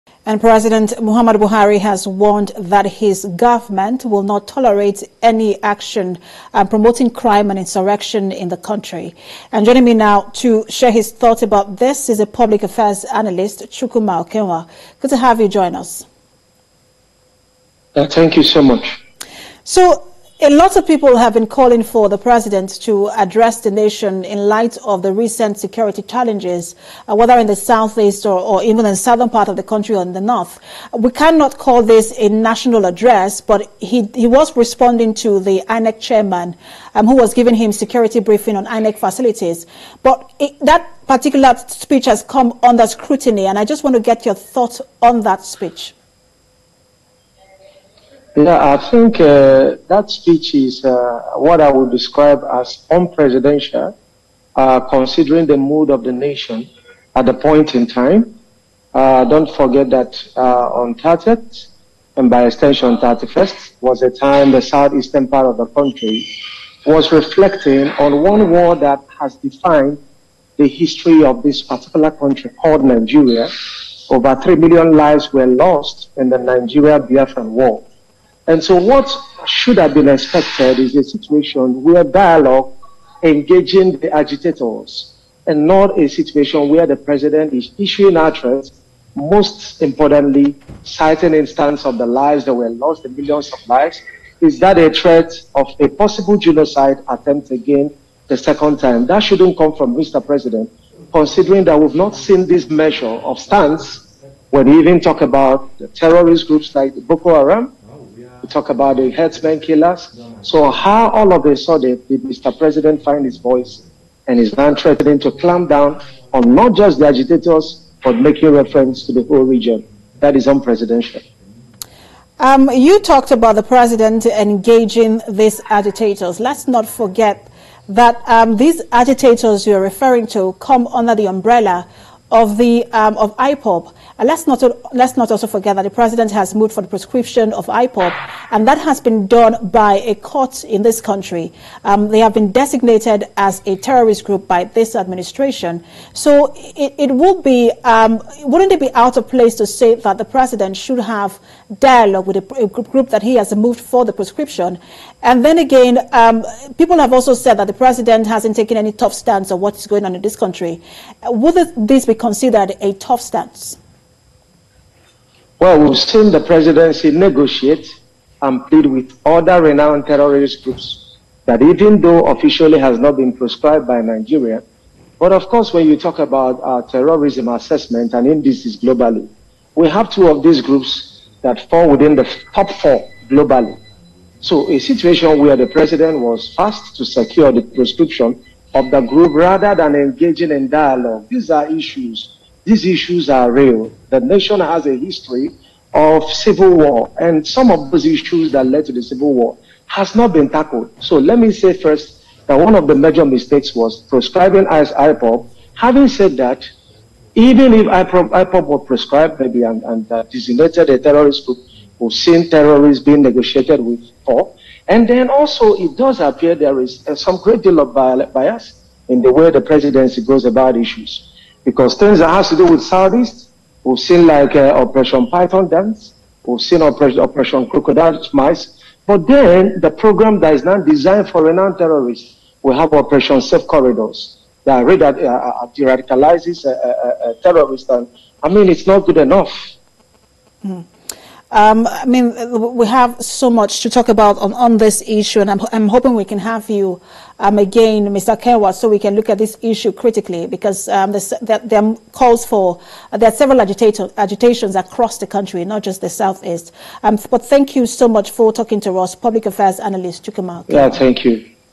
Public Affairs Analyst, Chukuma Okenwa, Speaks On Buhari's Message To Promoters Of Insurrection
In this interview, Public Affairs Analyst, Chukuma Okenwa, shares his views on Buhari's promise to deal with promoters of insurrection